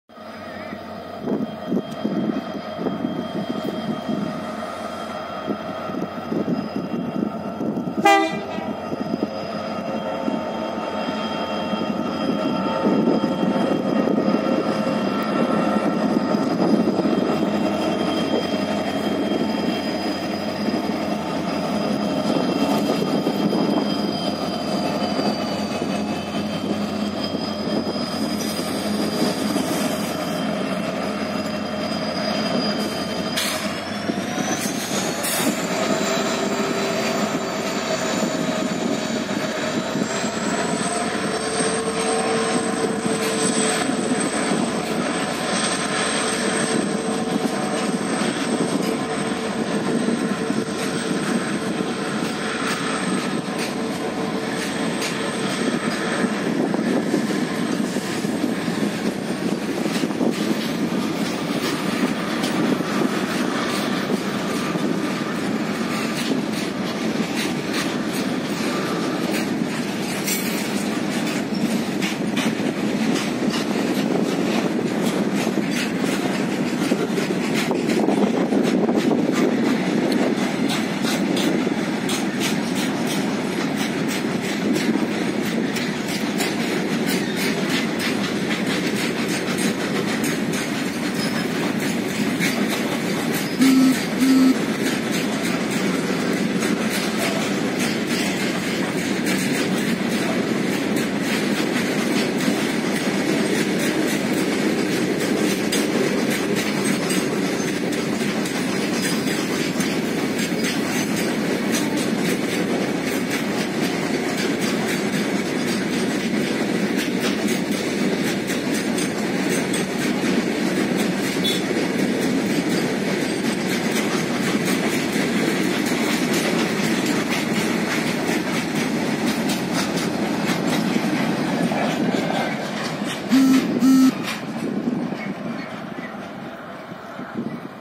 WDGs in Between Sulebhavi - Suladhal in Miraj - Londa Section
Doubling work in progress between Miraj - Londa Section